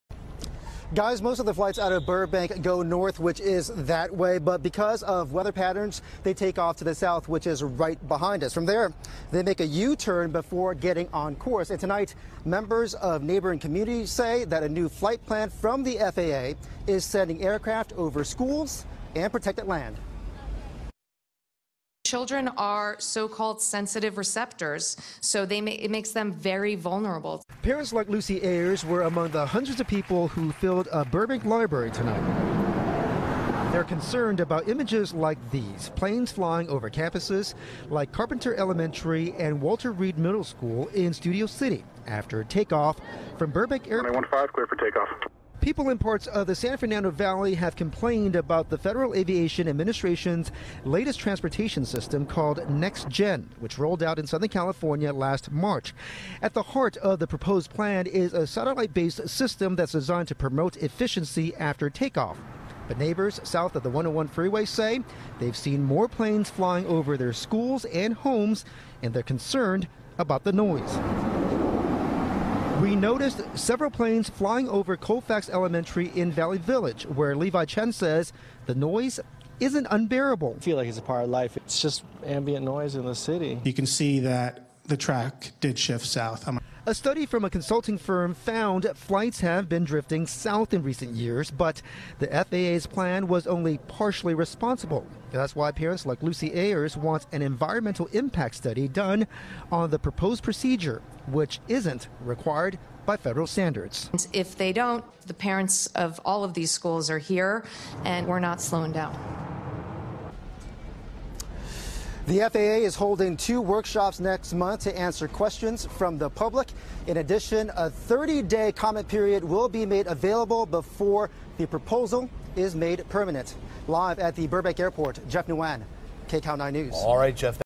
Concerns Over Shifting Flights Paths At Burbank Airport
A battle over Burbank Airport tonight. The FAA has been testing new flight paths into and out of the airport and people who live below them say the noise is unbearable. So a meeting was held tonight about the issues. Jeff Nguyen reports.